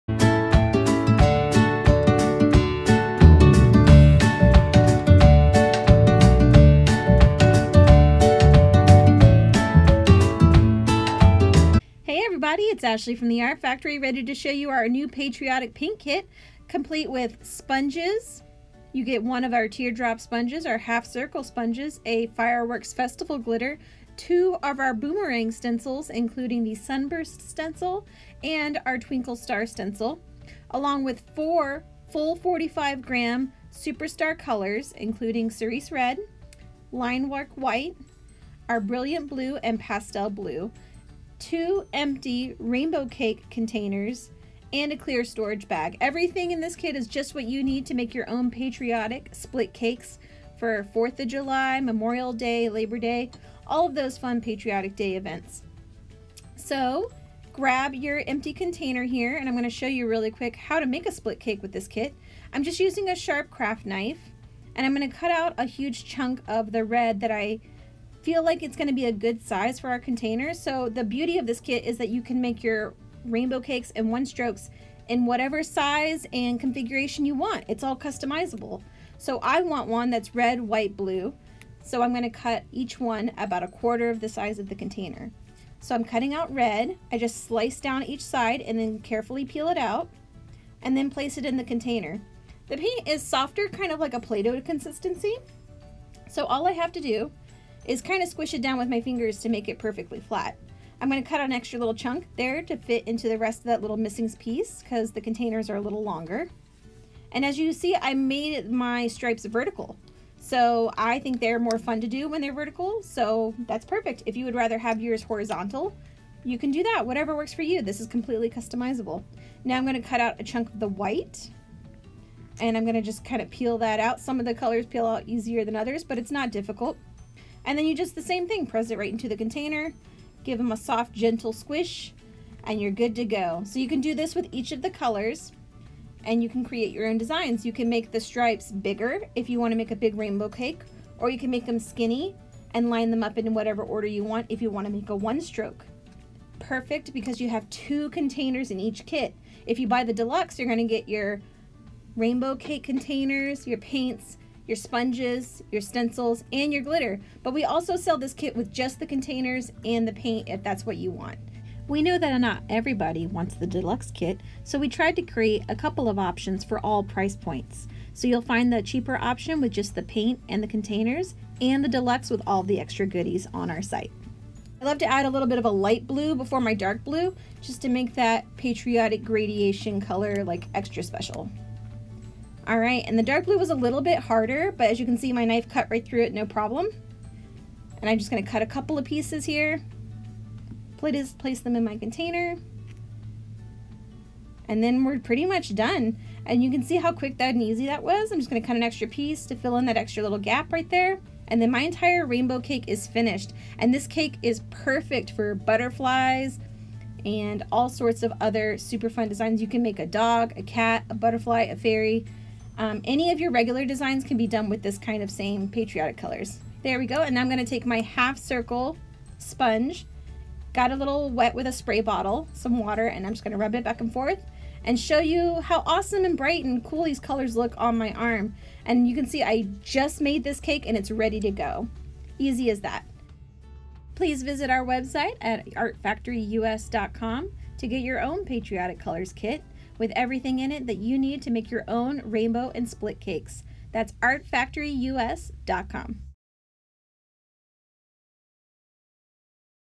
New Patriotic Face Paint Split Cake Kit by the Art Factory!
Check out the brand new Patriotic Face Paint Kit by the Art Factory! This DIY split cake kit includes everything you need to make your own red, white, and blue split cakes for your patriotic holiday events!

You can choose from our Deluxe Kit which includes:
-4 full 45 gram Superstar face paints
-2 empty split cake containers
-2 Boomerang stencils
-1 Festival Glitter
-2 face paint sponges
-1 storage bag

or the Basic Kit which includes:
-4 full 45 gram Superstar face paints
-2 empty split cake containers
-1 storage bag

Create your own rainbow cakes just the way you like them! Make your stripes horizontal or vertical, make the colors thick or thin, place your colors in whatever order you want!

Watch this video for a quick demonstration on how to make your own split cake.

Order the Patriotic Kit and all of your other face paint supplies today on our website!